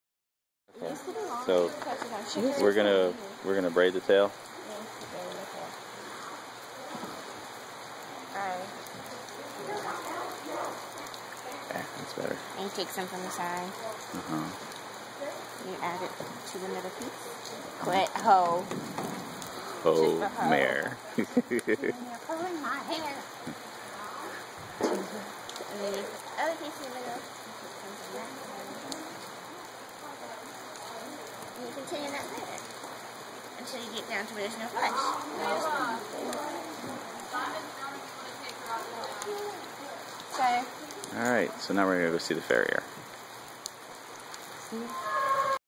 Chispa del Sol. Taking a horse by the tail.
We were waiting to get her bails done, so decided to get the hair braided while we waited.